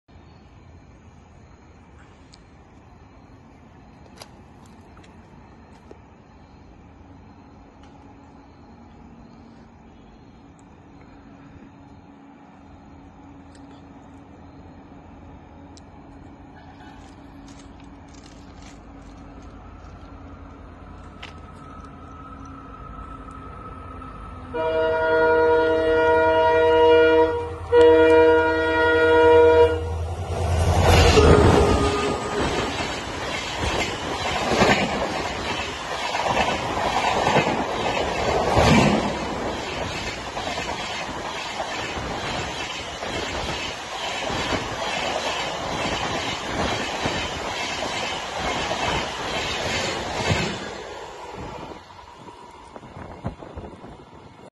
My close encounter with sbc belagavi SF express
Just to be unique I tried a low angel shot to feel the air of the train below platform level so I was prepared to take a shot from below platform level... train was also at good speed too.